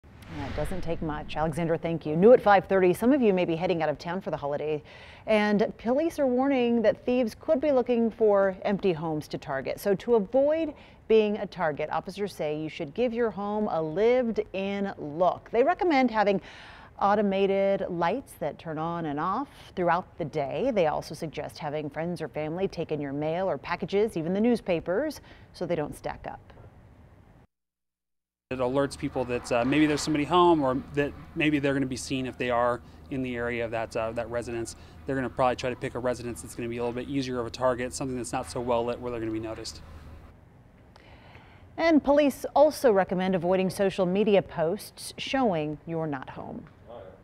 Protecting Homes From Thieves Over The Holidays
Police are warning some thieves will be looking to target empty homes.